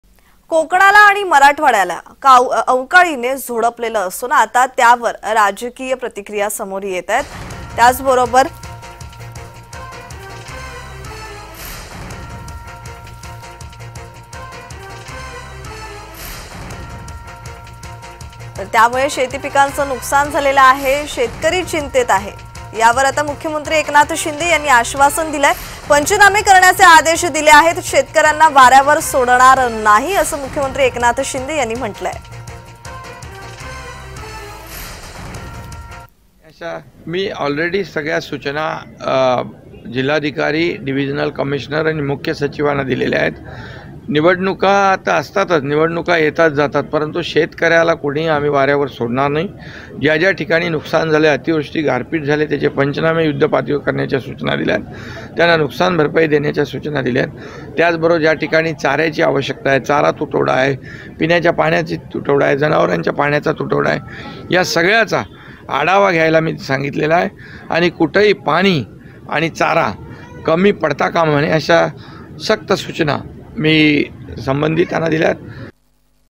Eknath shinde । 'शेतकऱ्यांना वाऱ्यावर सोडणार नाही' । Marathi News
'पंचनामे करण्याचे आदेश दिले आहेत'
'शेतकऱ्यांना वाऱ्यावर सोडणार नाही'
मुख्यमंत्री शिंदे यांचं आश्वासन